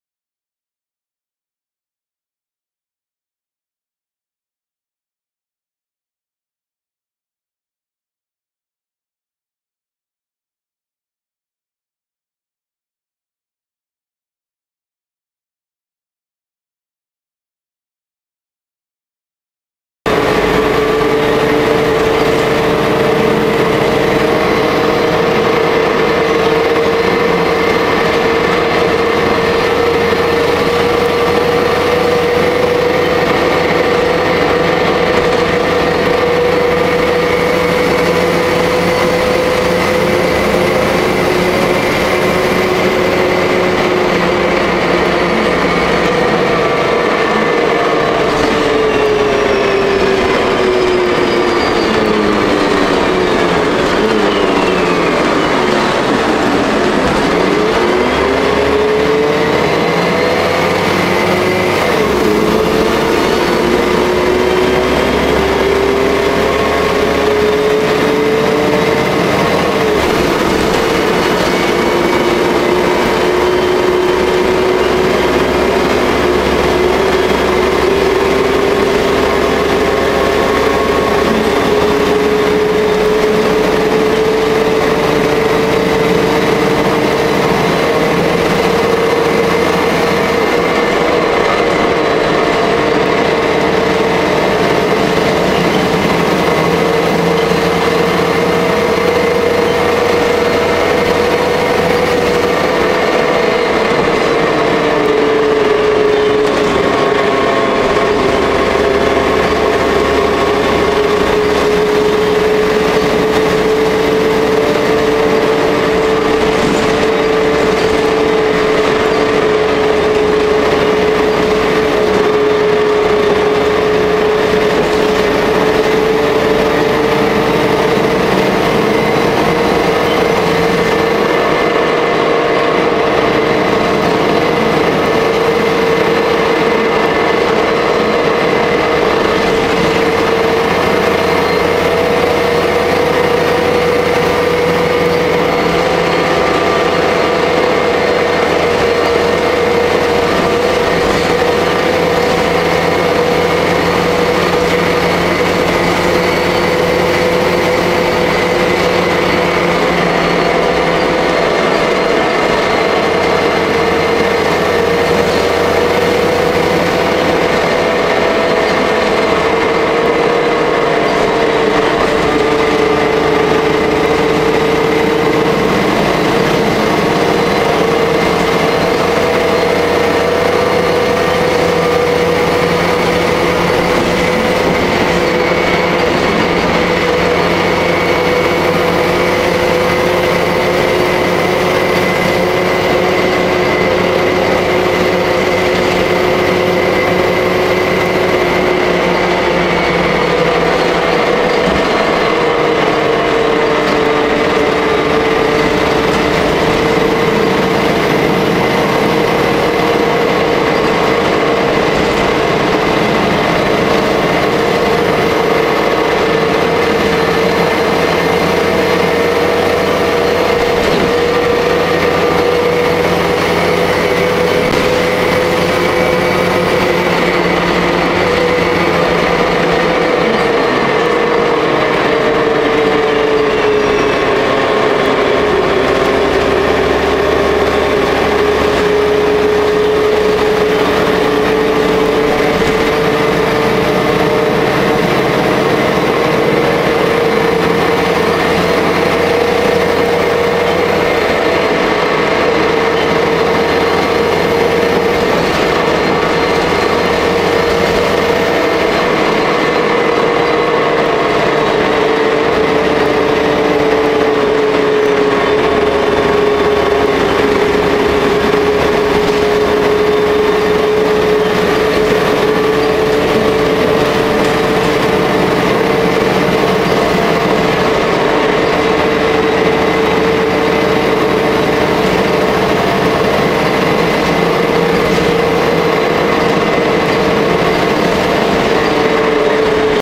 Jelcz L100 #55
MPK Nowy Sącz.
Linia 37 Nowy Sącz, Kazimierza Wielkiego - Bogusza
Jakoś trochę ucierpiała na poprawie drgań przez YouTube